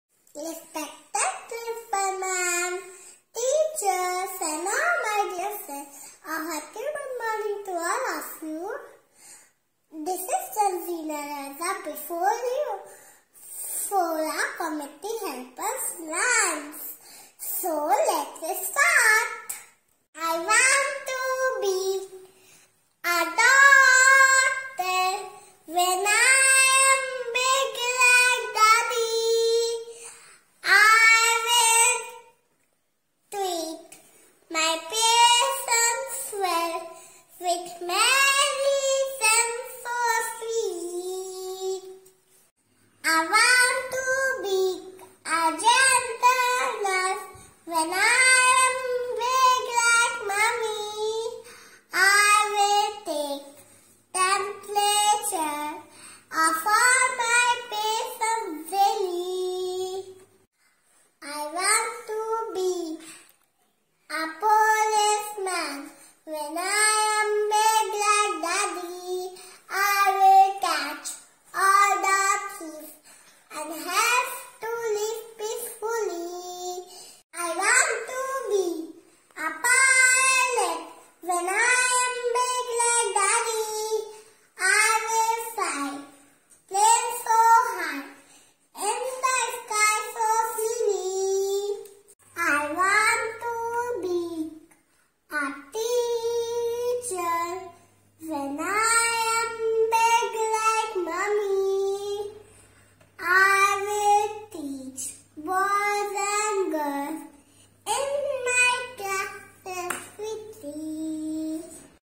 Community Helpers Rhymes#how to sing rhymes community helpers
community Helpers Rhymes video or audio with good singing way and tone, dress up different different for different communities helpers